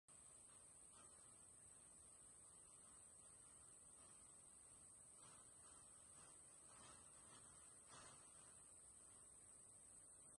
Lo que es demasiado no puede ser bueno ​⁠@cliRgiana
Después de tantos días de lluvia...
Río Fluvià.
Sant Joan les Fonts. Garrotxa. Girona província. Catalunya. España. 11/10/2020.